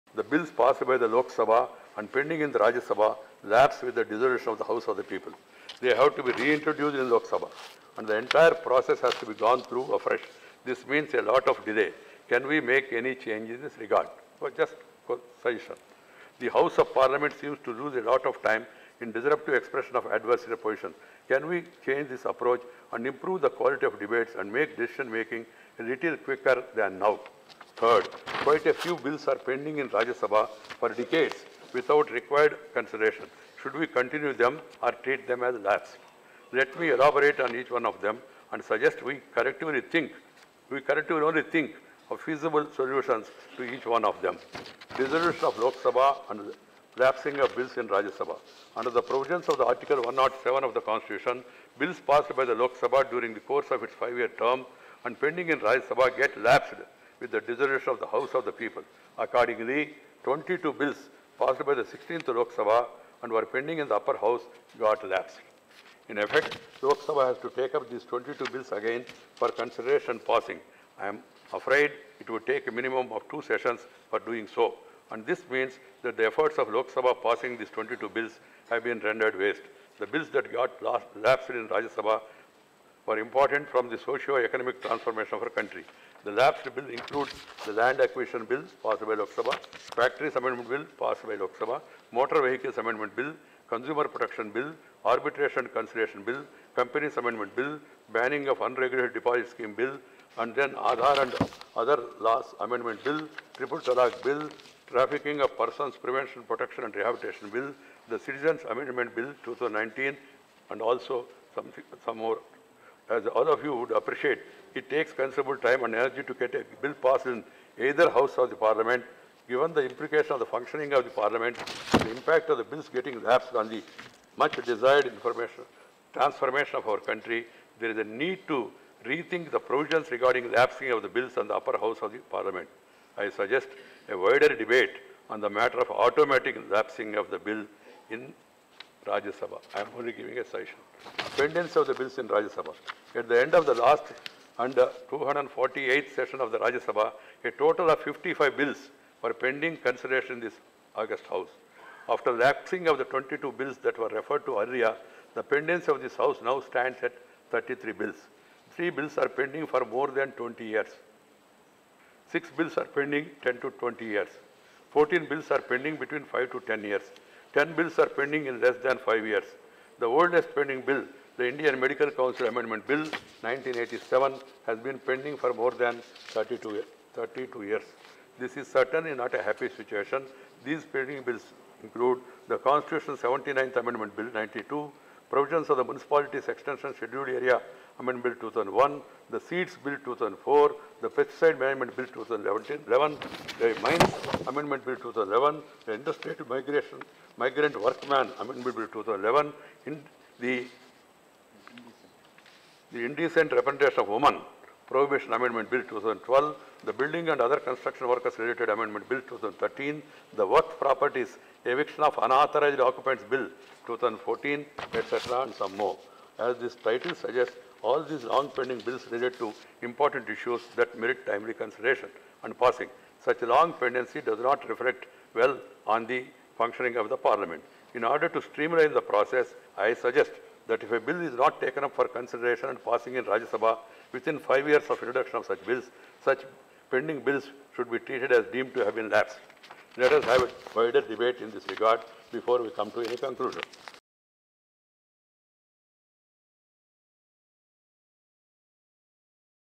Save bills from lapsing: Vice President Naidu's appeal to Rajya Sabha MPs
Vice-President of India and Chairman of the Rajya Sabha, Venkaiah Naidu appealed to members of the House to try and make its functioning more efficient. He cited the example of bills pending in the Upper House of Parliament for decades, while those which were passed by the 16th Lok Sabha lapsing in Rajya Sabha after the former was dissolved. V-P Naidu said that there was a need for debate on how to prevent lapsing of bills passed by LS and pending in RS, while bringing in a system for automatic lapsing of bills pending in Rajya Sabha for multiple years.